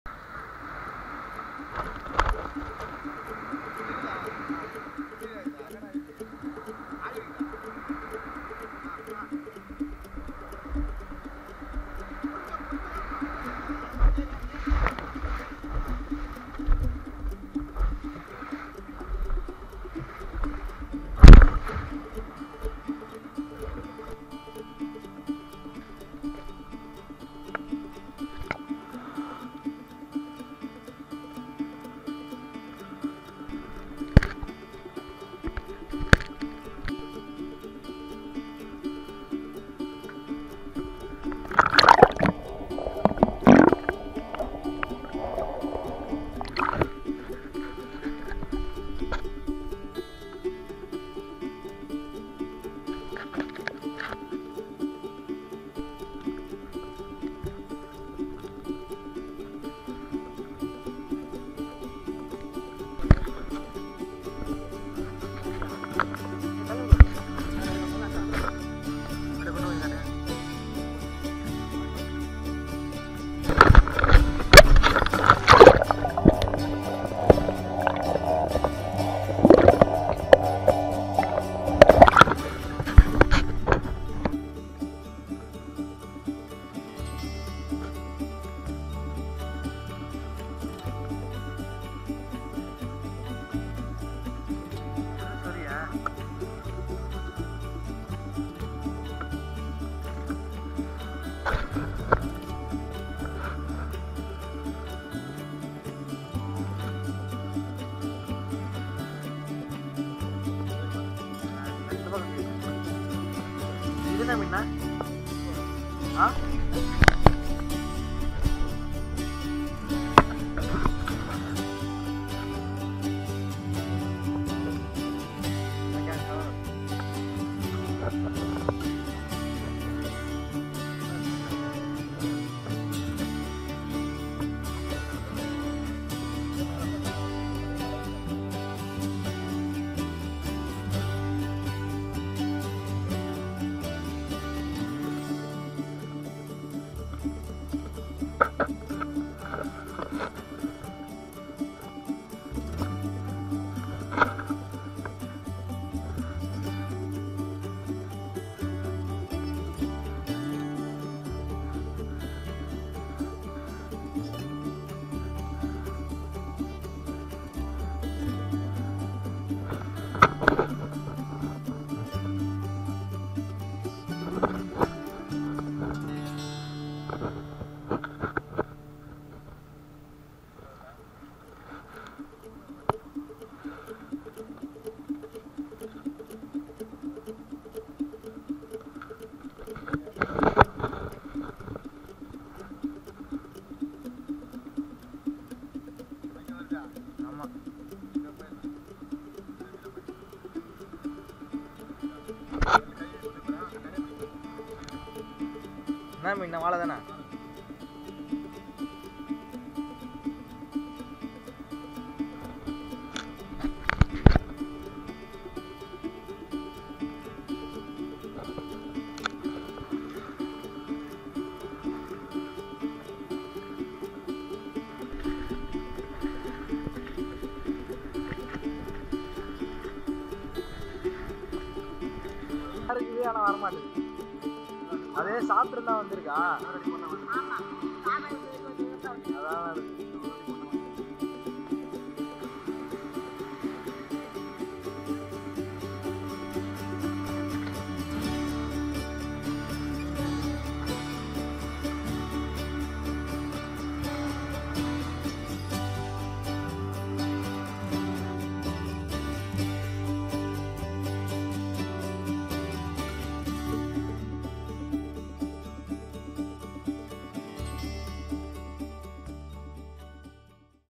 Traditional Indian Fishing | Wow! amazing Native fishing | Meenavan The Fisherman
Fishing is very dangerous job. This video shows the life of Indian Fisherman. There is lot of poisonous fishes and snakes also lives in seas. Fisherman should be more careful in their job. Please avoid swimming in sea or ocean when you sea jelly fishes in it. They are most dangerous and their sting are venomous. Fisherman face day to day many struggle to led a normal life.